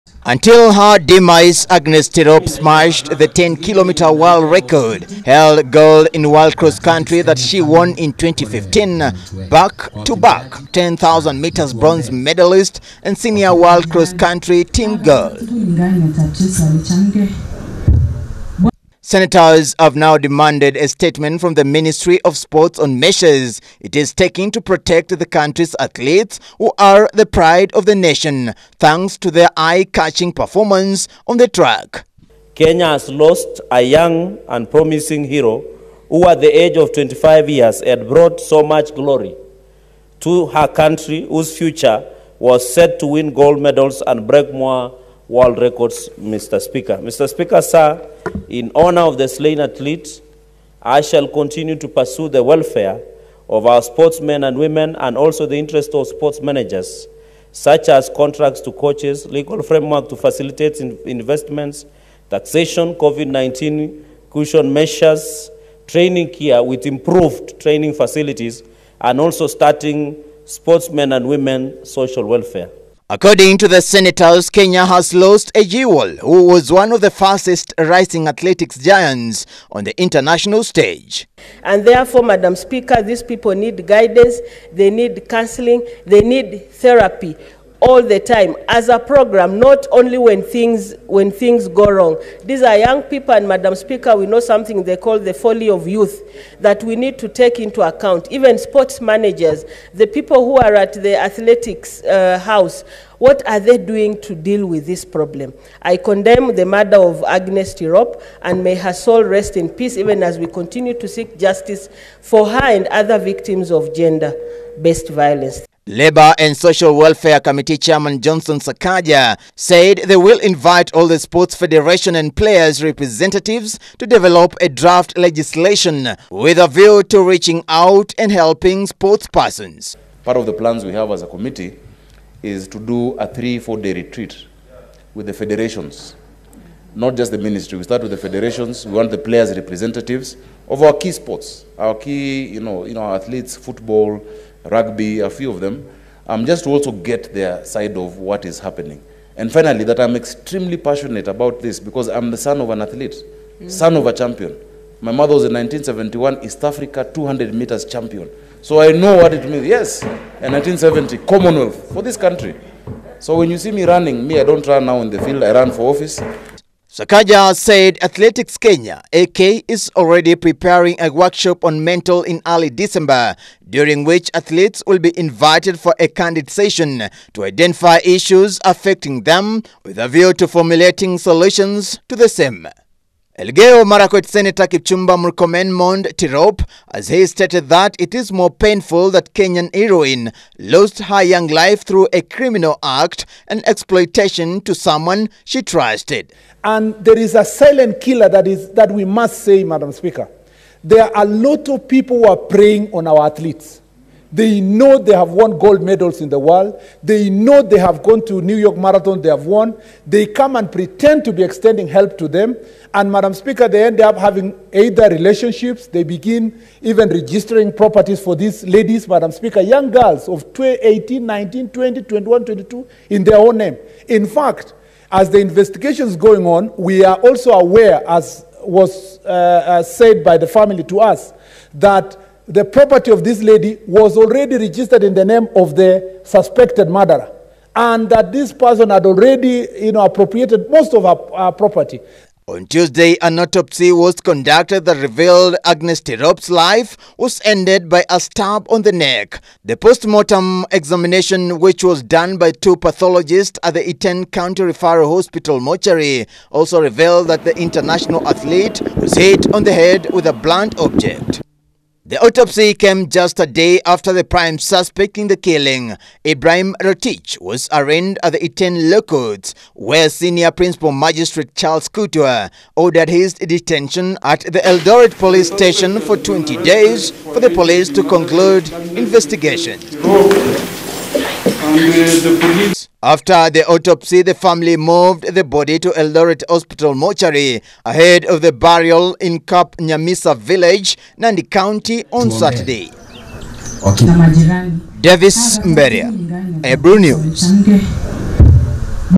Senators Suspended Business To Pay Tribute To Agnes Tirop
Senators Suspended Business To Pay Tribute To Record-Breaking Long-Distance (10km) Runner Agnes Tirop Who Was Last Week Stabbed To Death At Her Home In Iten, Eldoret.Nandi Senator Samson Cherarkey Who Moved The Motion Said Kenya Has Lost A Jewel Who Was One Of The Fastest-Rising Athletics Giants On The International Stage.